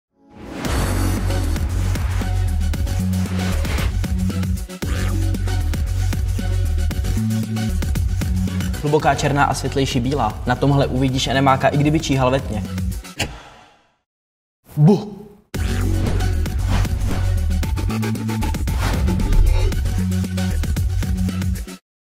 Mirko "Orinn" Baumann | Samsung C27RG50
Reklama na monitor v podání Mirko "Orinn" Baumanna 😁